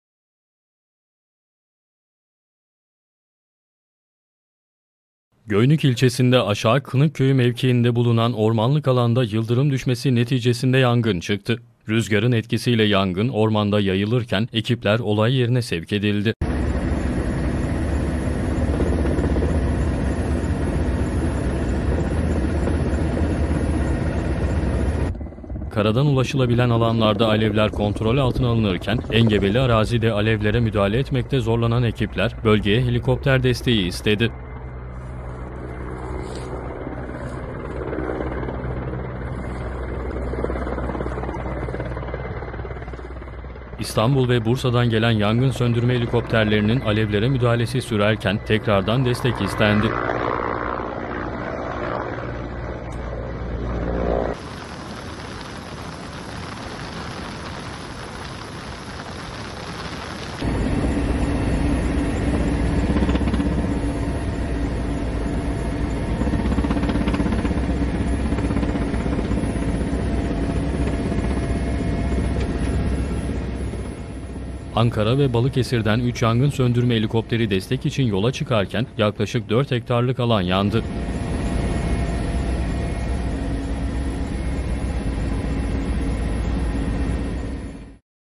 YILDIRIM DÜŞMESİ SONUCU YANGIN ÇIKTI : 3 YANGIN SÖNDÜRME HELİKOPTERİ DAHA DESTEĞE GÖNDERİLDİ
Bolu’nun Göynük ilçesinde yıldırım düşmesi neticesinde dün akşam meydana gelen yangına müdahale sürüyor. Bölgede 2 helikopterle söndürme çalışmaları sürerken Ankara ve Balıkesir'den 3 yangın söndürme helikopteri desteğe gönderildi.